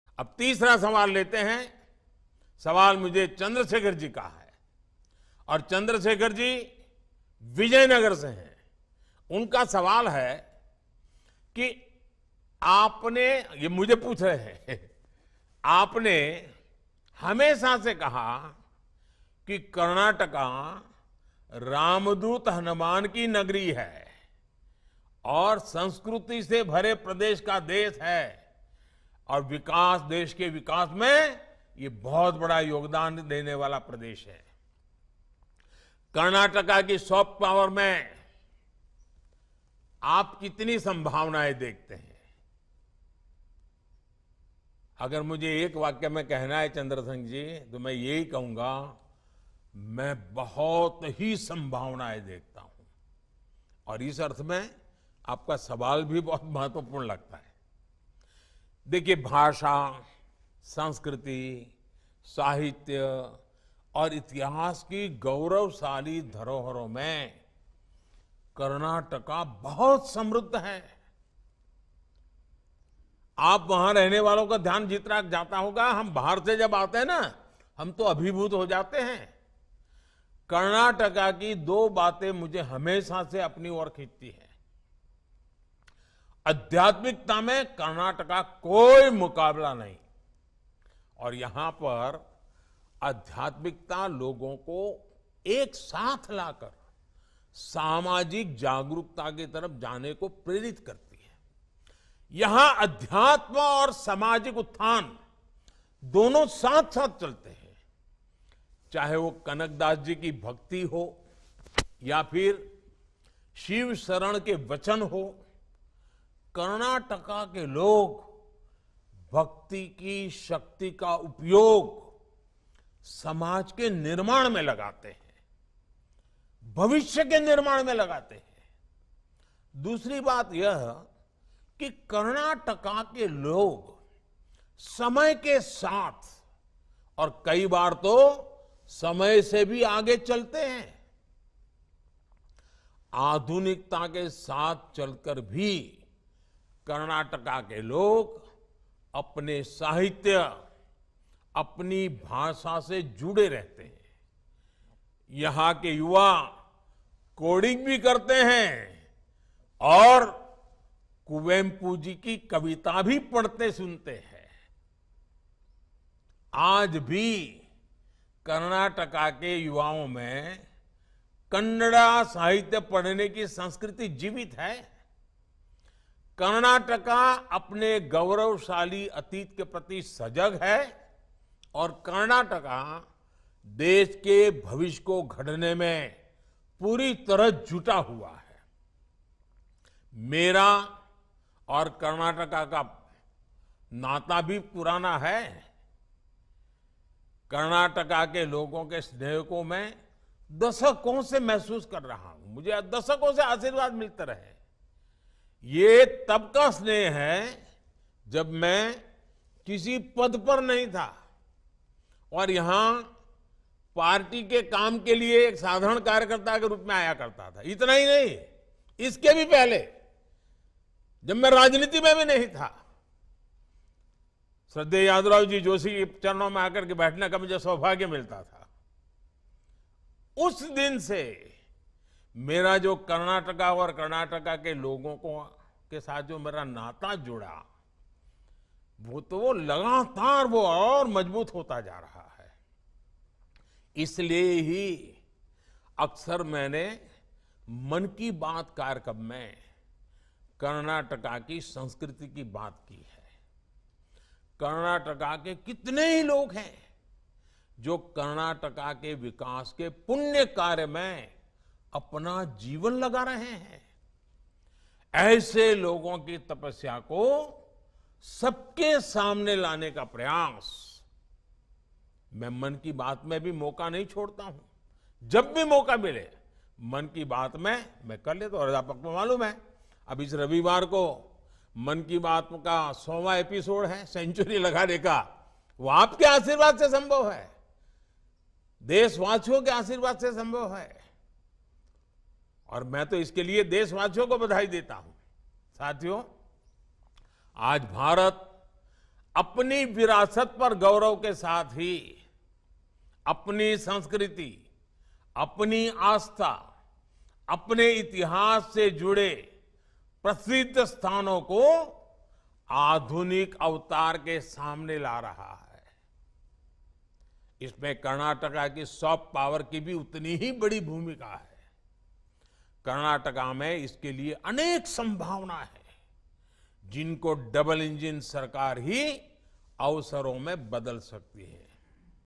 Vijayanagar के चंद्रशेखर के सवाल पर मोदी जी ने बताया- Karnataka का देश के विकास में क्या योगदान है?
Prime Minister Narendra Modi addresses Party Karyakartas and voters of Karnataka via video conferencing.